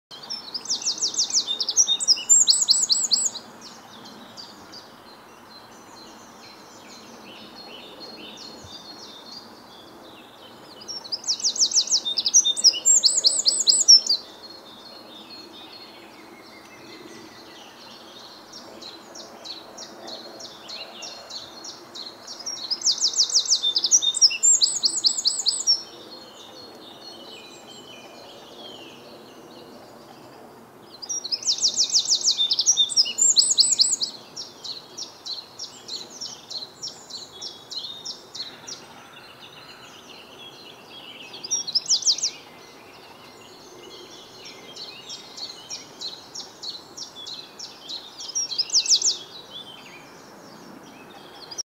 Wren singing with visible breath on a chilly morning
Reading UK, 9 April 2022